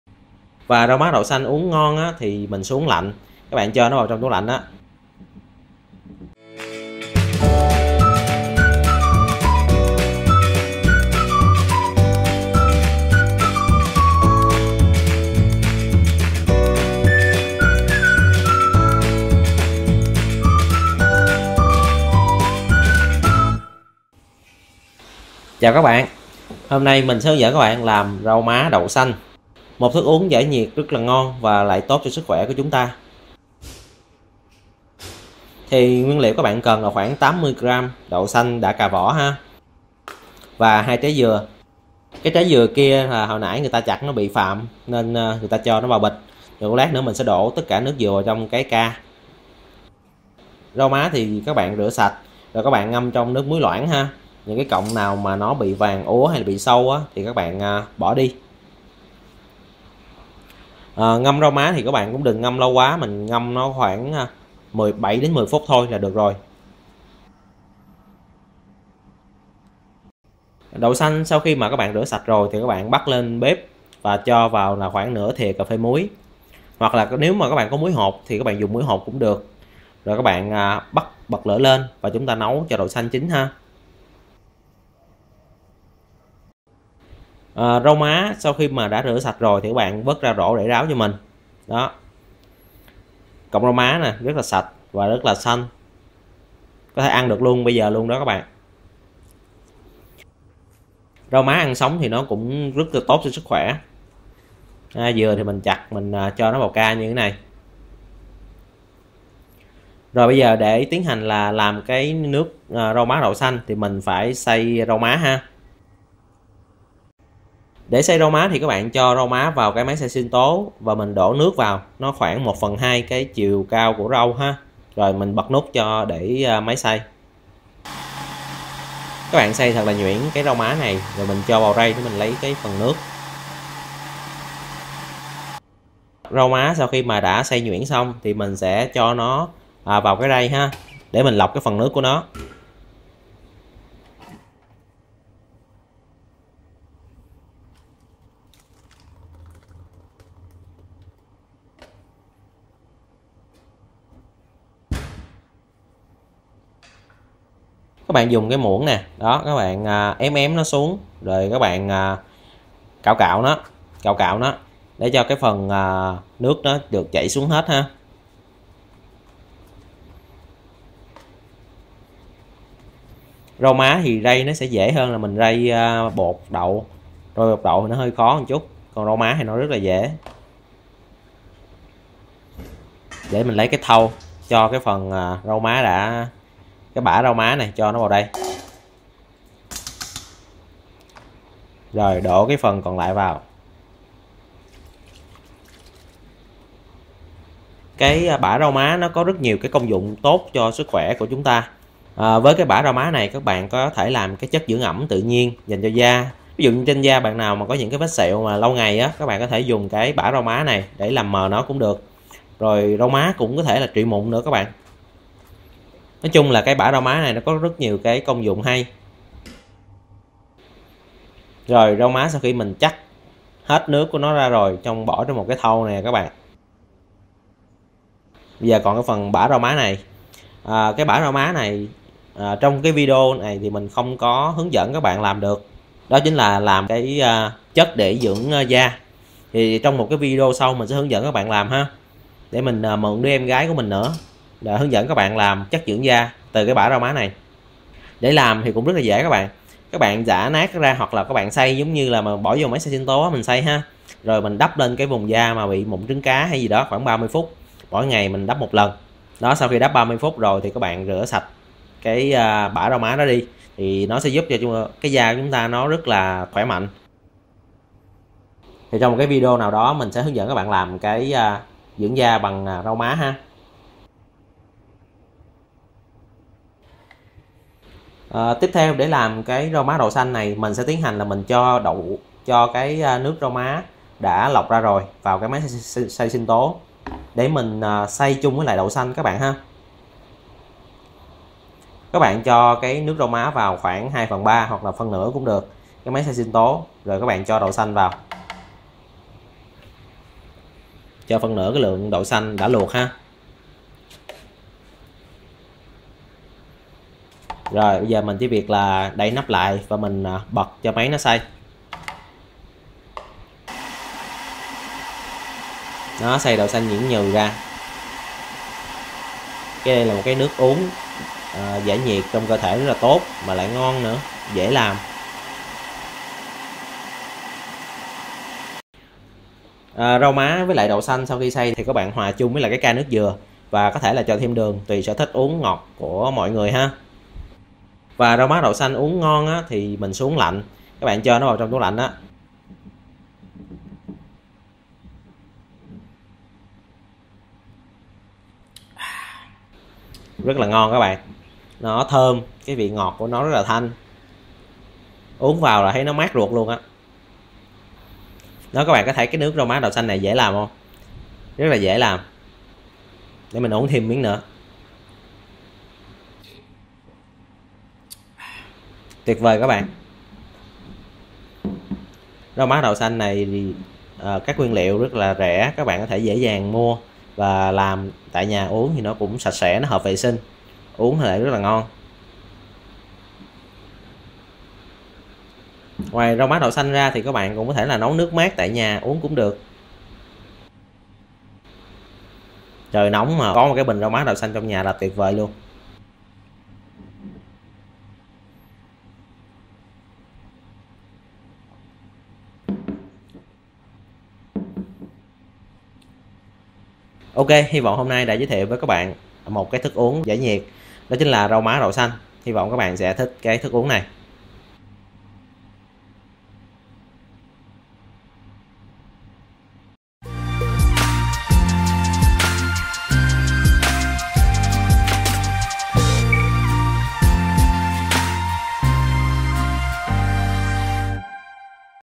Cách làm nước Rau Má Đậu Xanh Ngon tại Nhà - Cười Khúc Khích
Nắng nóng kéo dài thì hãy xem cách làm nước rau má đậu xanh của mình chia sẻ trong Video hôm nay.

Rau má dùng với đậu xanh, nước dừa là một trong những cách làm rau má đậu xanh ngon nhất, được rất nhiều quán xá vỉa hè, giải khát bán hàng ngày.

Cách làm nước rau má đậu xanh vô cùng đơn giản và dễ làm. Với những nguyên liệu dễ tìm mua và giá thành hợp lý, bạn có thể làm nước rau má đậu xanh cho gia đình mình vào những dịp cuối tuần, vừa ngon, vừa đảm bảo vệ sinh.

Nhiều bạn không biết rau má có tác dụng gì? Công dụng của rau má trong việc trị mụn, dưỡng da? Mình sẽ có Video hướng dẫn các bạn làm nhé.Trị mụn bằng rau má vừa đẹp tự nhiên vừa tốt cho làn da.